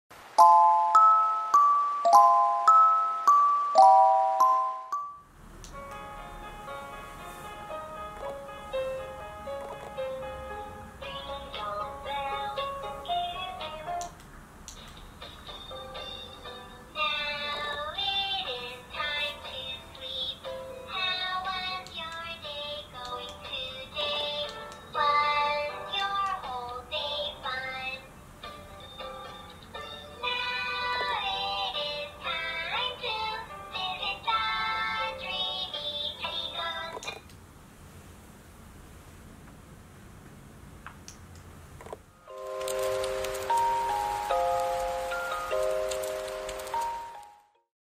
Cuteoy Light up Musical Unicorn Plush LED Singing Stuffed Animals Adjustable Volume ,14’’
Equipped with colorful LED lights inside, this cozy singing light up musical unicorn toy will glow in the dark when turn on and will comfort you. It also helps both kids and adults relax when taking a nap in the afternoon or accompanying those scared of dark during bedtime, which will help reduce anxiety and help them self-soothe in order for a better relax.